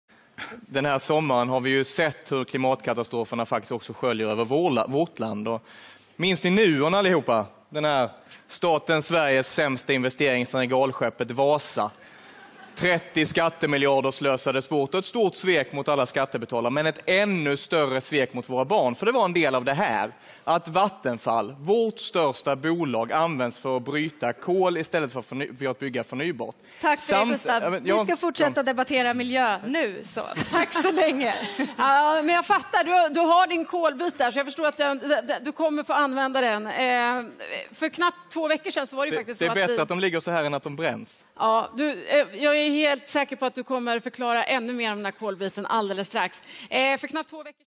Gustav Fridolins kolbit
Gustav Fridolin viftar med en kolbit i Afonbladets partiledardebatt.